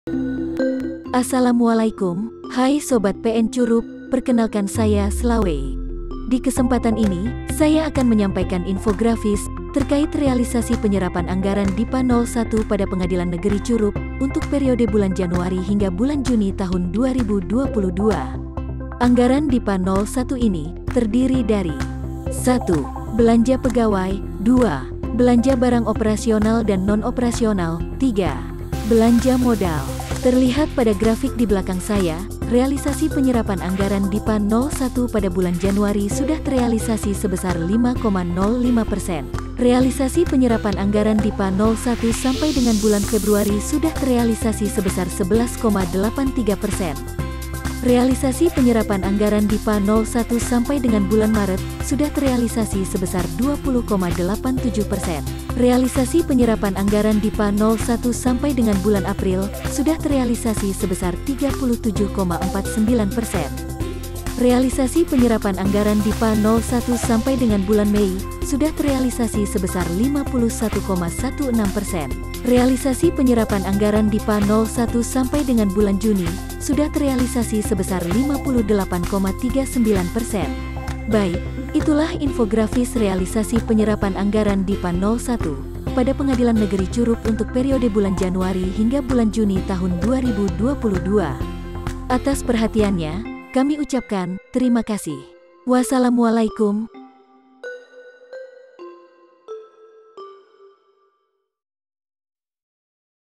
Infographis Realisasi DIPA 01 Bulan Januari - Juni Tahun 2022
Slawei adalah Virtual Asisten yang akan menemani Sobat PNCurup dalam berinteraksi ataupun mendapatkan informasi layanan dari Pengadilan Negeri Curup. Di kesempatan ini, Slawei akan menjelaskan terkait realisasi anggaran DIPA 01 dari Bulan Januari hingga Bulan Juni Tahun 2022.
Informasi yang disajikan bersumber dari Bagian Umum dan Keuangan Pengadilan Negeri Curup.
FYI, Slawei diambil dari Bahasa Rejang yang artinya Perempuan.